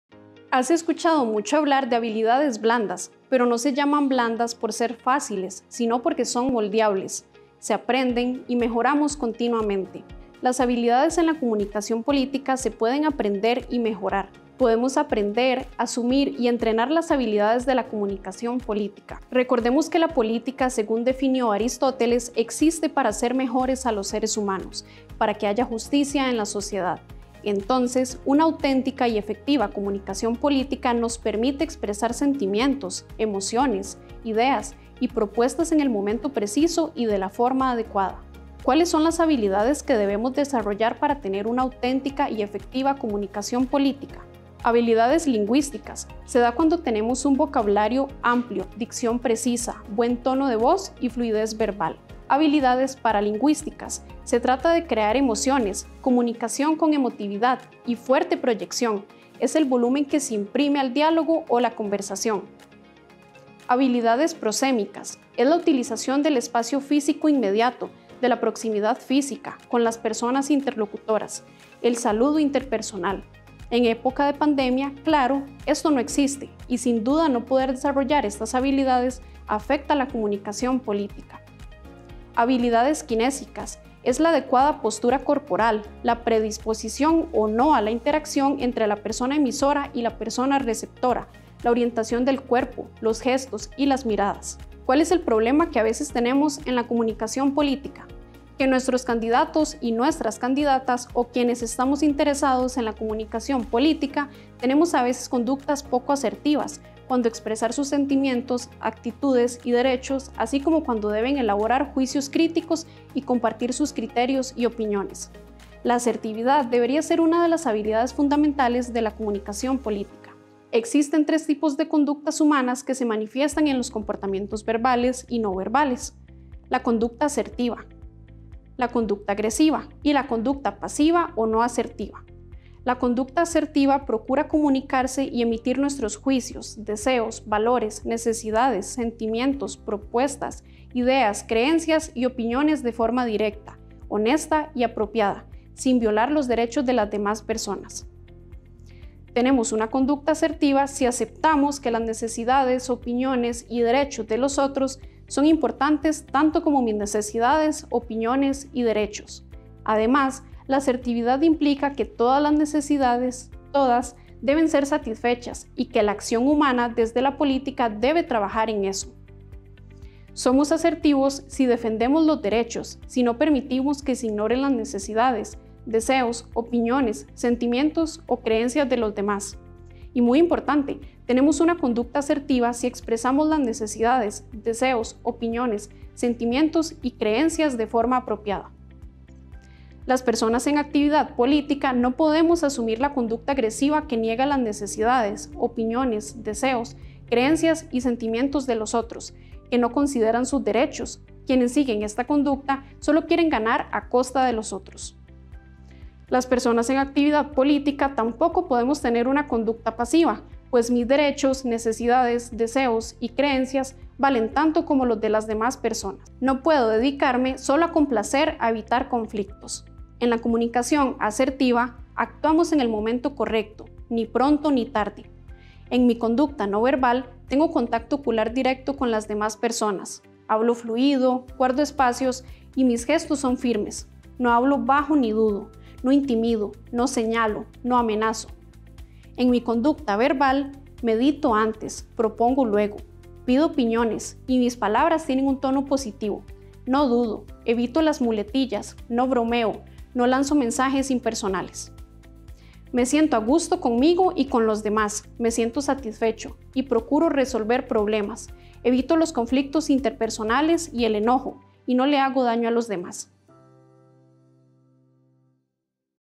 Habilidades en la comunicación
Para tener una auténtica y efectiva comunicación política hay que tener y desarrollar varias habilidades.